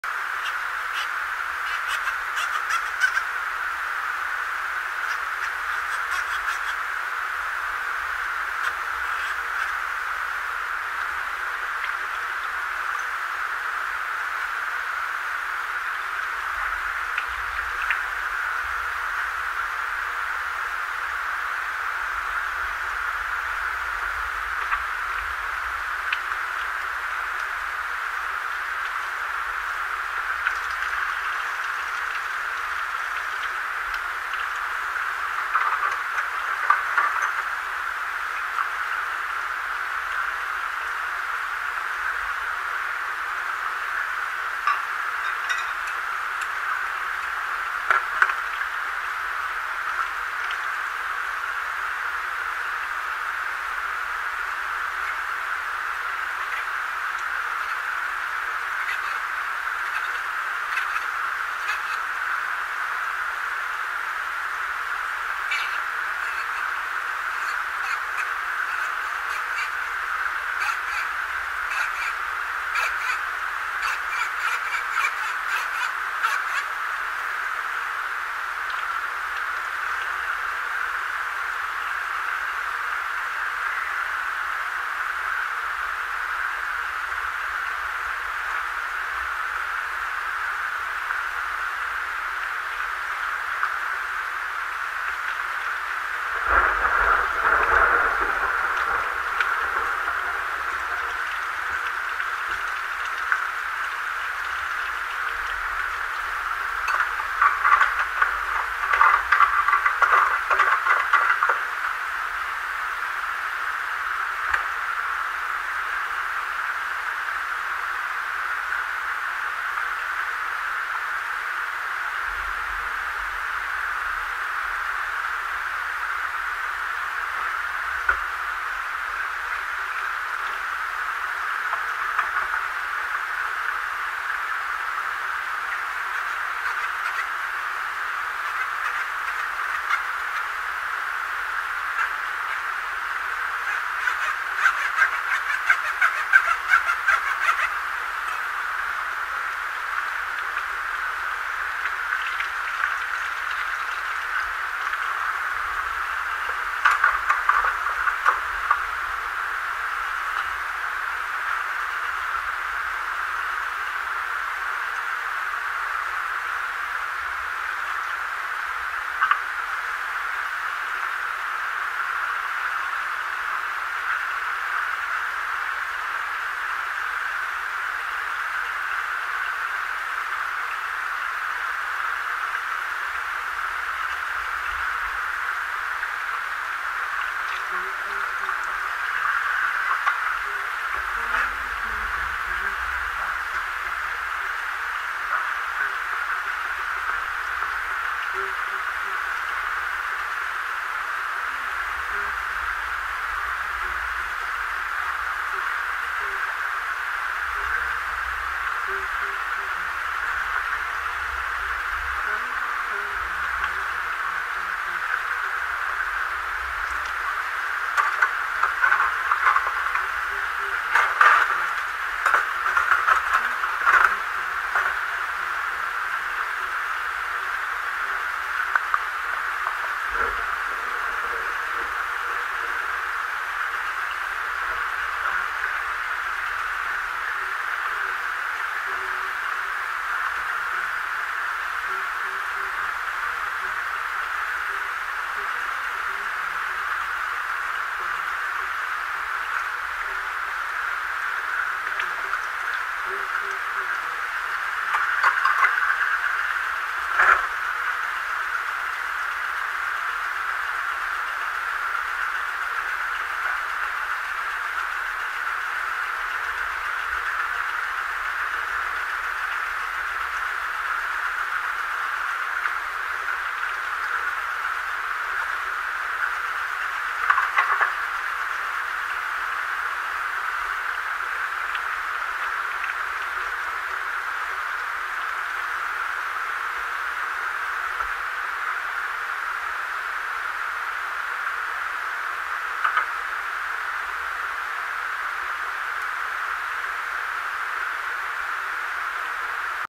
Washing Dishes Through a Storm.mp4
Just washing Some Dishes With The TV on Durin’ a Thunder Storm, it was a Nasty Storm Too.